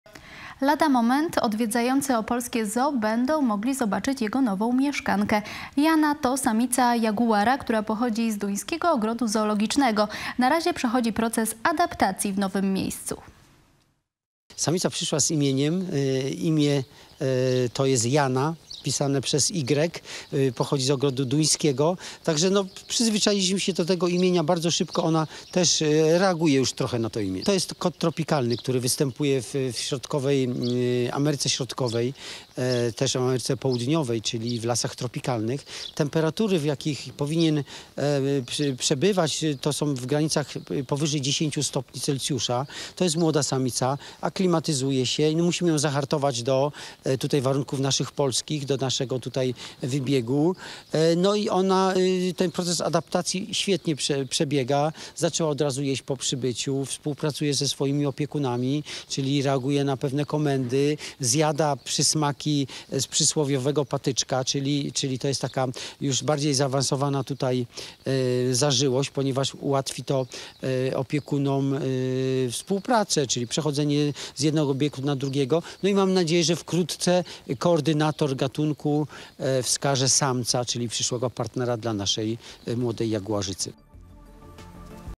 Nowy mieszkaniec opolskiego zoo. To samica jaguara
Lada moment odwiedzający opolskie zoo będą mogli zobaczyć jego nową mieszkankę. Yana to samica jaguara, która pochodzi z duńskiego ogrodu zoologicznego. Na razie przechodzi proces adaptacji w nowym miejscu.